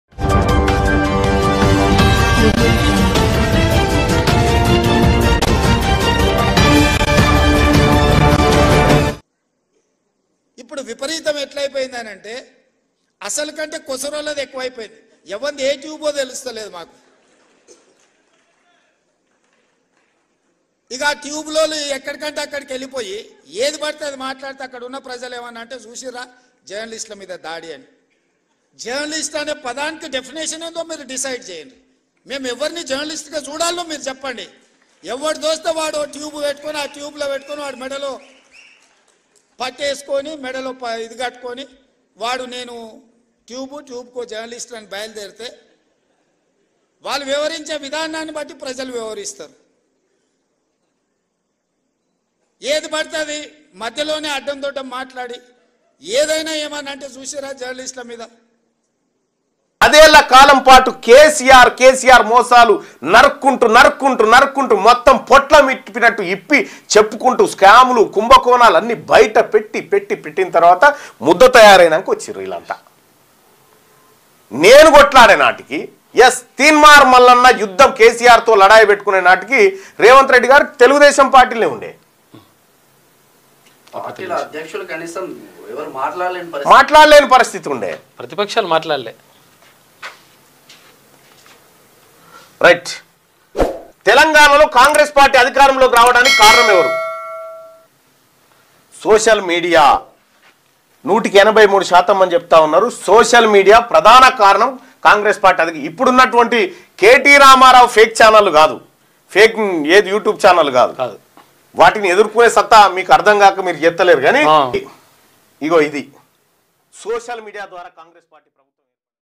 ||సీఎం రేవంత్ రెడ్డి జర్ణలిస్టులపై చేసిన వాక్యాలకు మల్లన్న కౌంటర్||
నేను కెసిఆర్ పై యుద్ధం చేసినప్పుడు రేవంత్ రెడ్డి TDP లో ఉన్నారుnk24tv